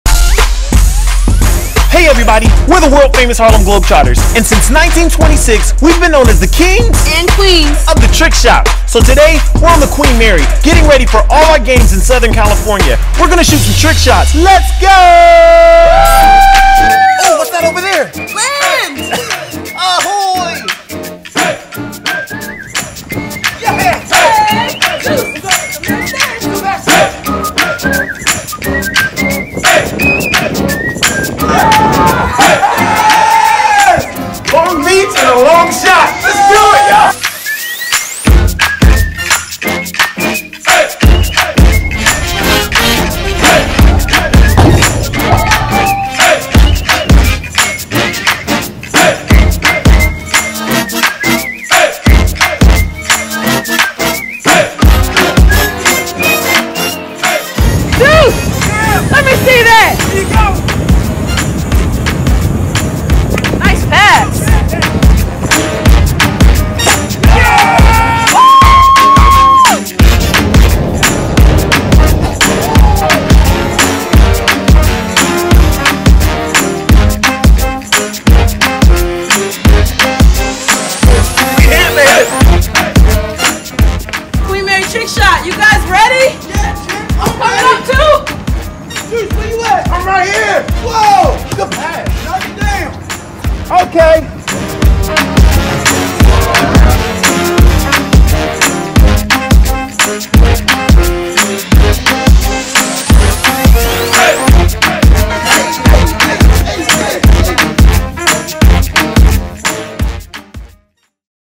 Incroyables trick shots des Harlem Globetrotters à bord du Queen Mary
Les Harlem Globetrotters continuent de marquer l'histoire en réalisant d'incroyables trick shot à bord du fameux Queen Mary. Ils enchaînent des passes et des tirs au panier sensationnels.